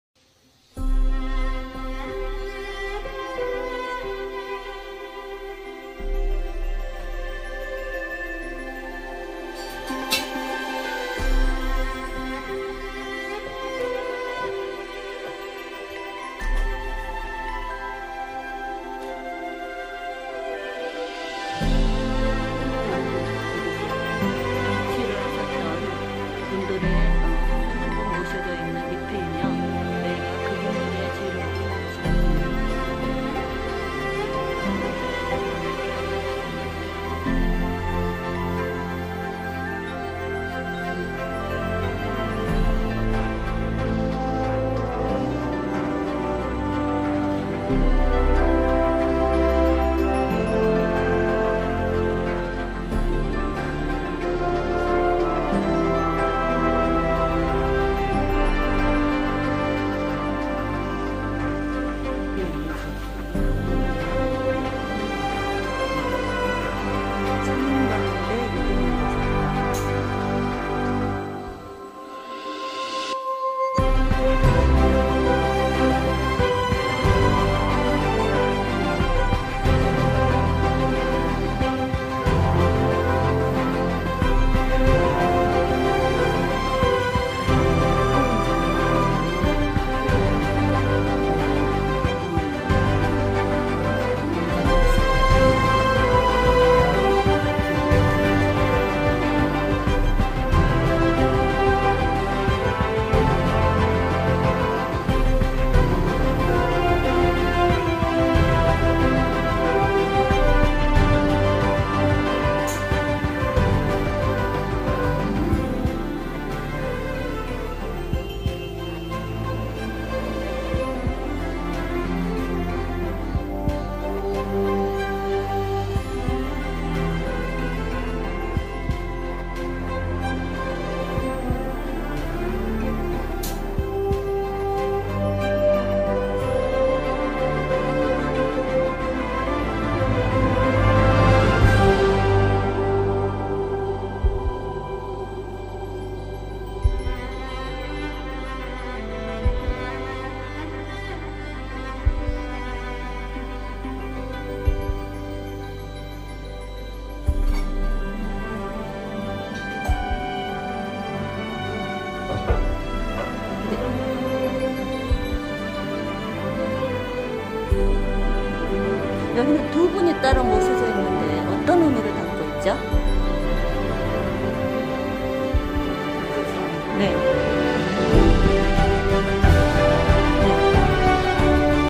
초혼각지
충남 공주시 반포면 동학사 1로 462 초혼각지는 숙모전과 삼은각 동계사가 있다.